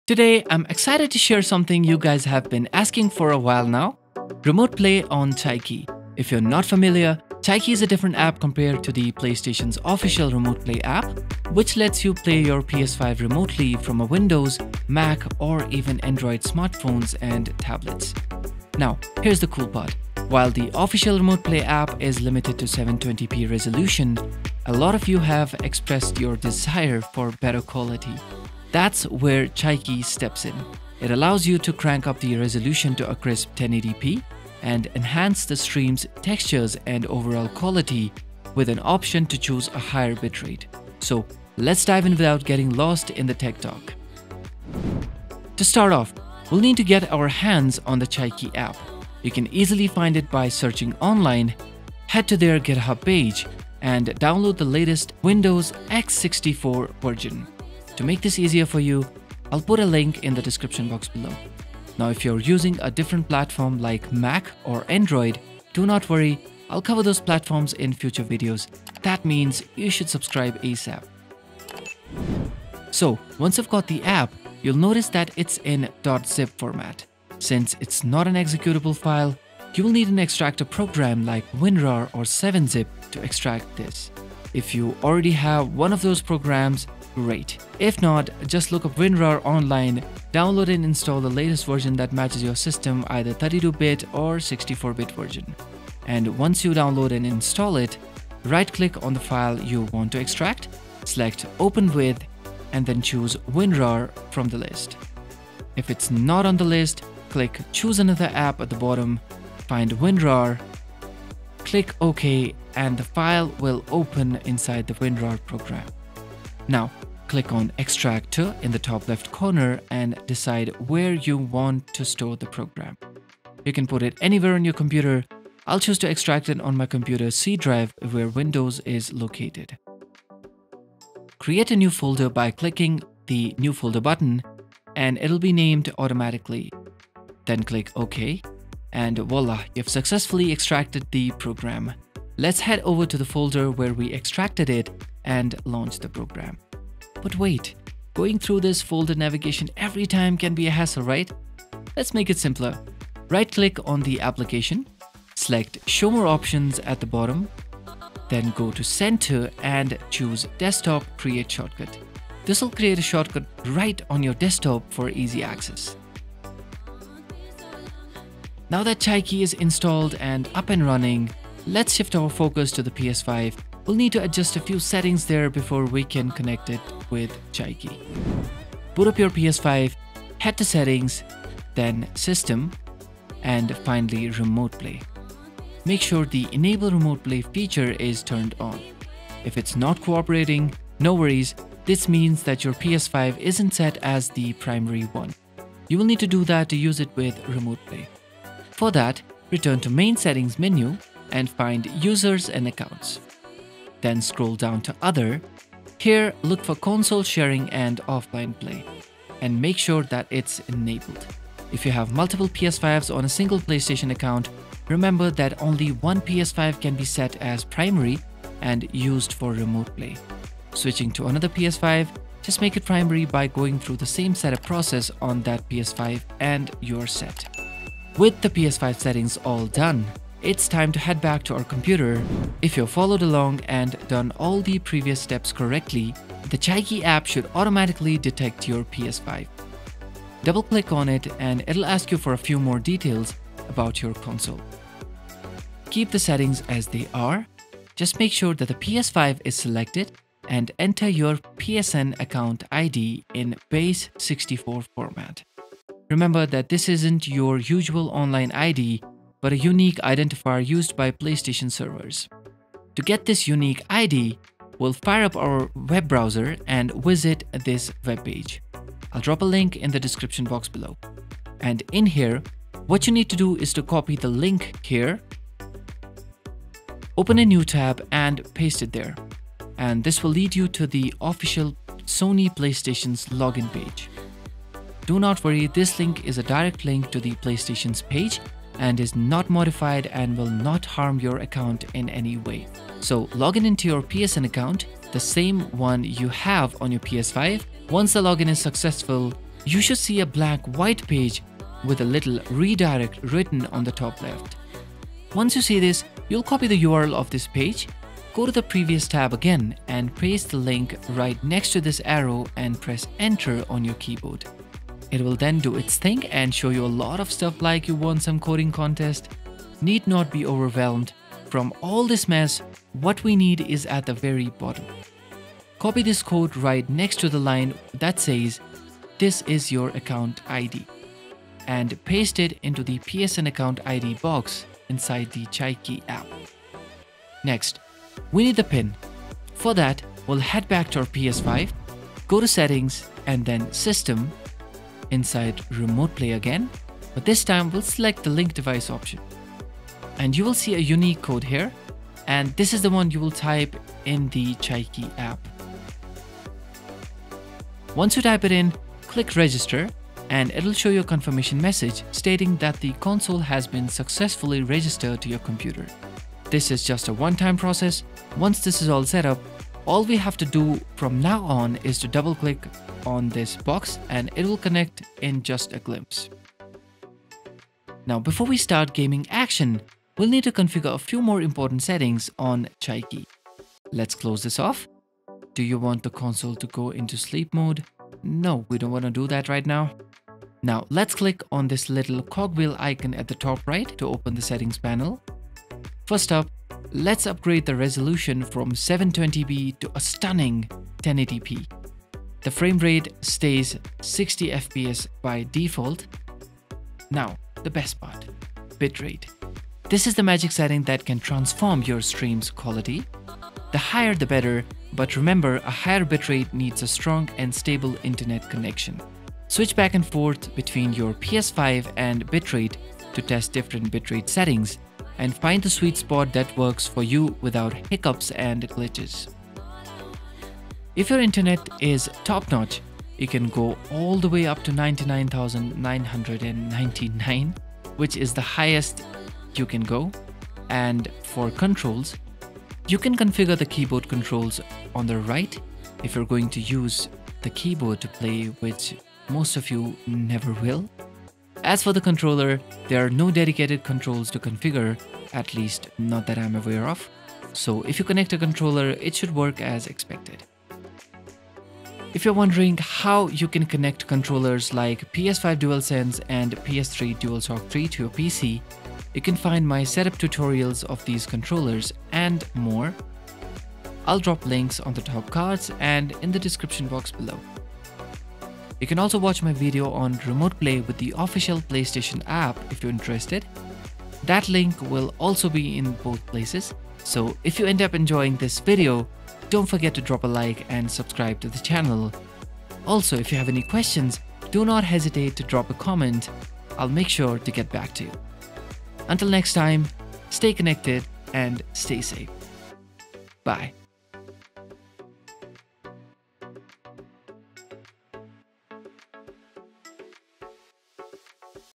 PS Remote Play | Remote Play PS5 with Chiaki on PC | Chiaki Setup & Tutorial
In this video, I'll Explain How to Setup PS Remote Play with Chiaki on PC. With Chiaki you'll be able to Remote Play your PS5 on PC with better Resolution ,Textures and upto 60FPS. This is a Step by Step Setup Tutorial on Remote Playing PS5 with Chiaki on PC.

OTHER LINKS